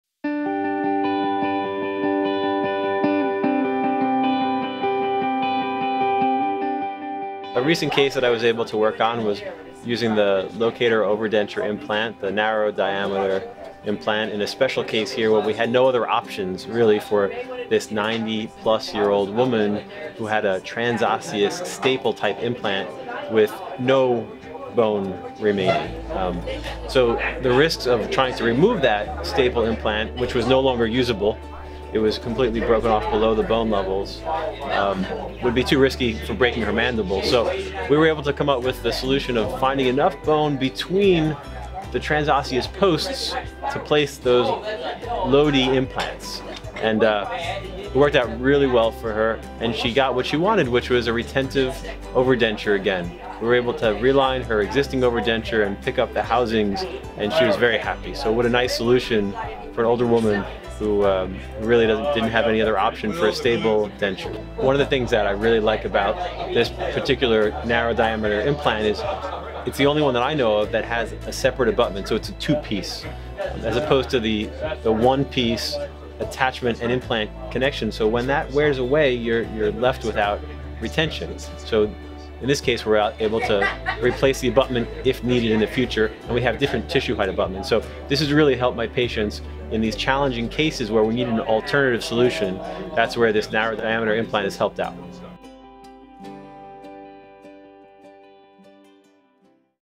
Zest LOCATOR Overdenture Implant System Testimonial - Dr. Anthony Prudenti
Dr. Prudenti discusses how he was able to utilize the LOCATOR Overdenture Implant system to treat an elderly patient faced with common anatomic limitations and get her back to enjoying day to day life again.